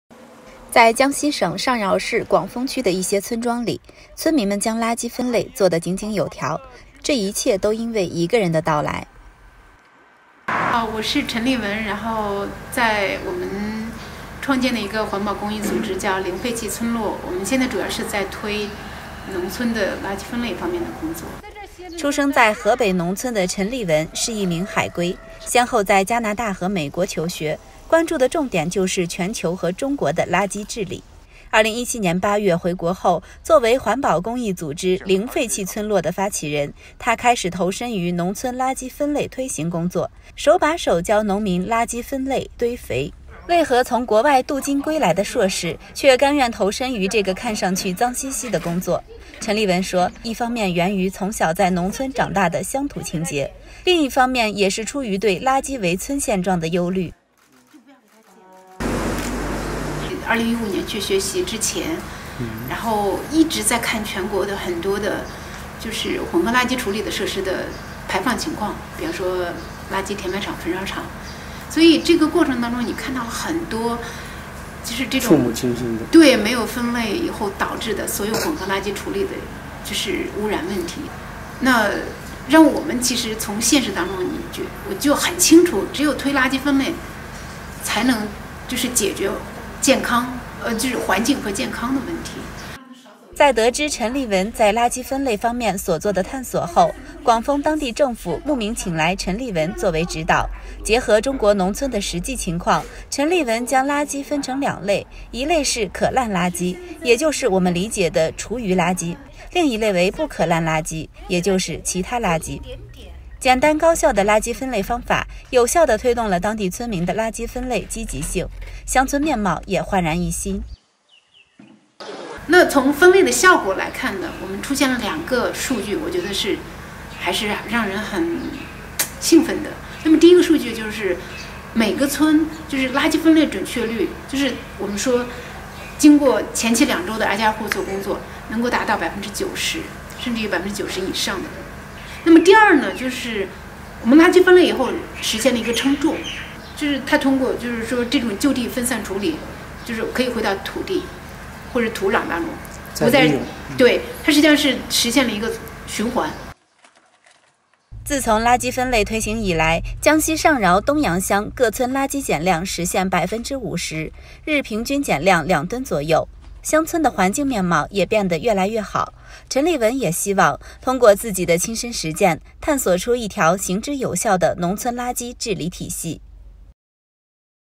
海归硕士江西农村捡垃圾 发起“零废弃村落”环保组织
在江西省上饶市广丰区的一些村庄里，村民们将垃圾分类做得井井有条，这一切的变化都因为一个人的到来。出生在河北农村的陈立雯是一名海归，先后在加拿大和美国求学，关注的重点就是全球和中国的垃圾治理。2017年8月回国后，作为环保公益组织“零废弃村落”的发起人，她开始投身于农村垃圾分类推行工作，手把手教农民垃圾分类、堆肥。